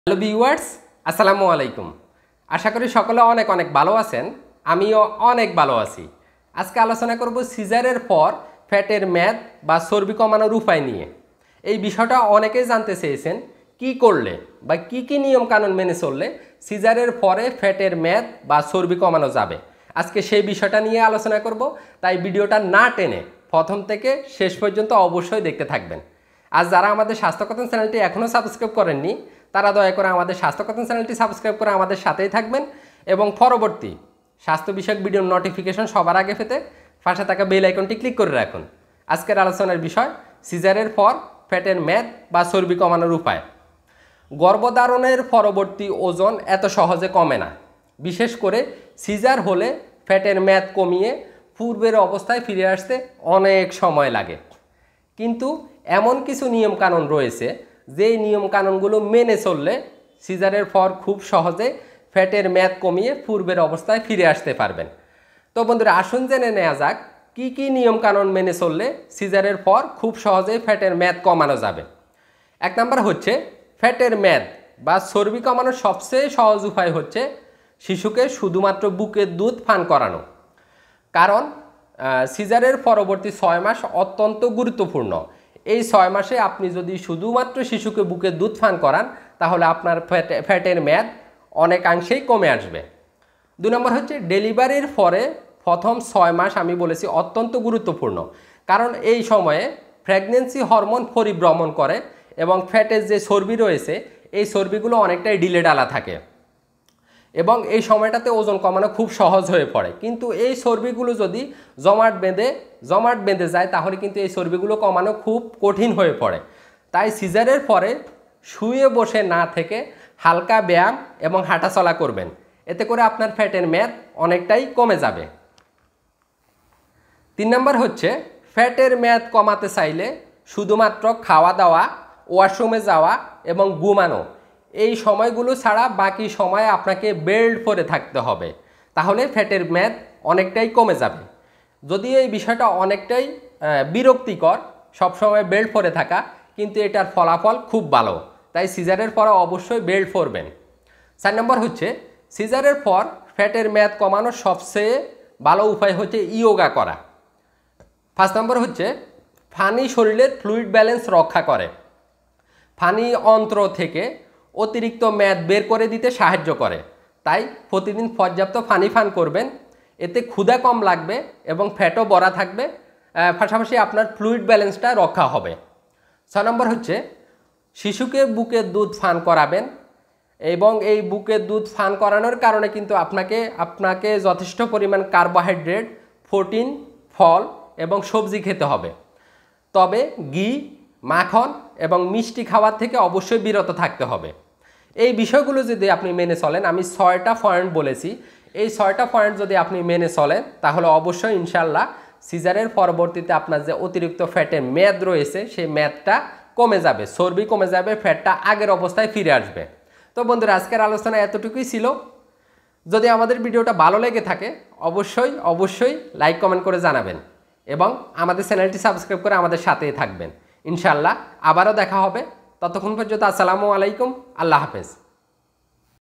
পেটের মেদ/চর্বি কমানোর উপায়।সিজারের পর পেটের মেদ/চর্বি কমানোর উপায়।
আজকে আলোচনা করবো সিজারের পর কি কি নিয়ম কানুন মেনে চললে পেটের মেদ বা চর্বি কমানো যাবে।

পেটের_মেট/চর্বি_কমানোর_উপায়